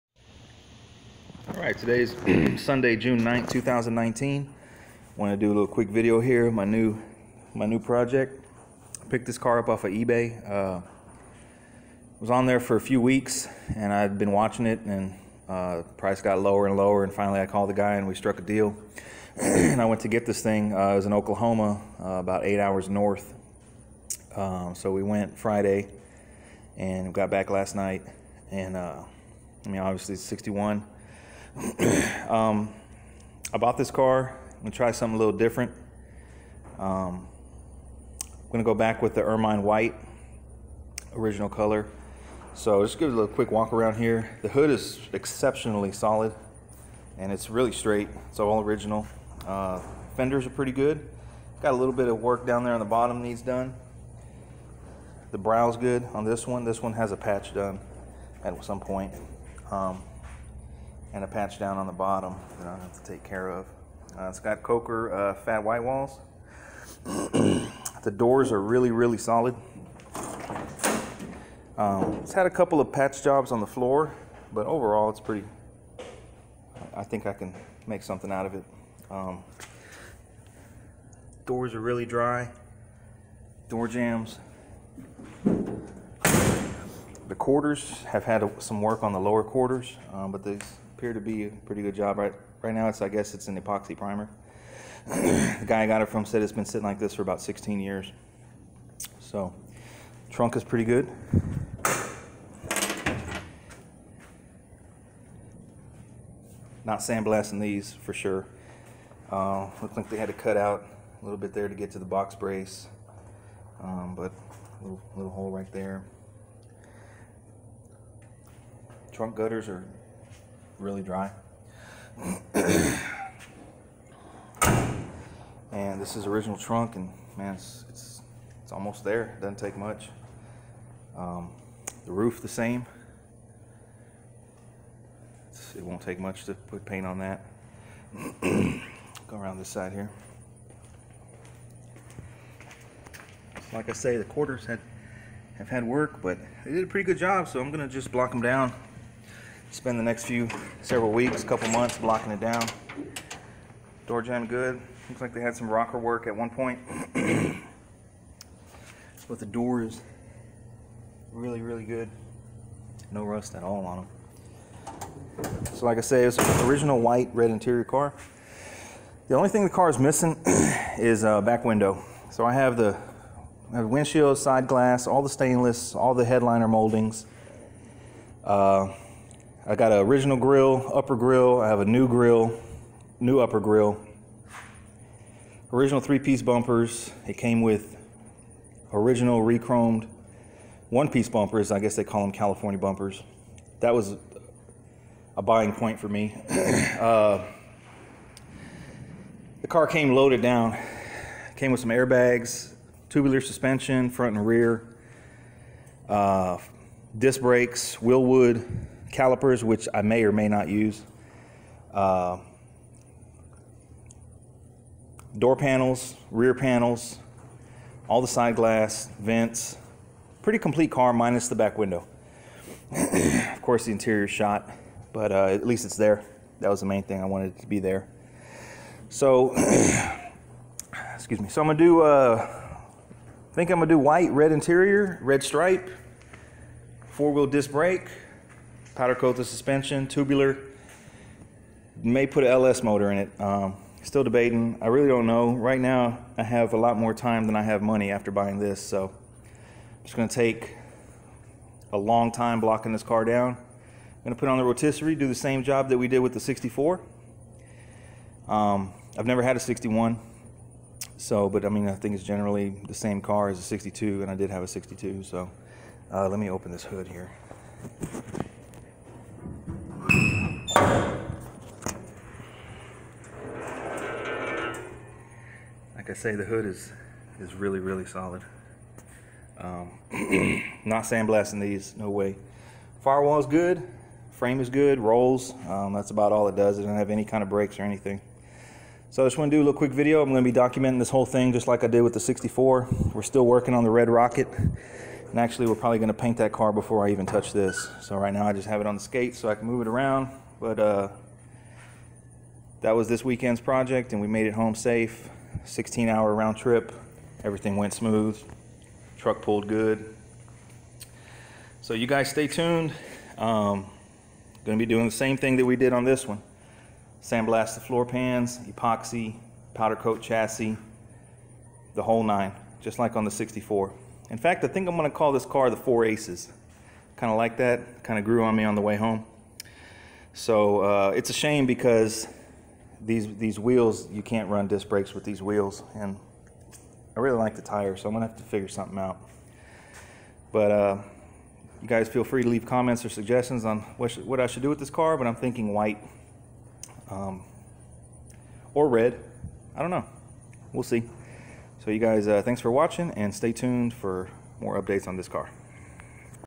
1961 CHEVROLET IMPALA WALK AROUND.
Walk around of the new project.  1961 bubble top!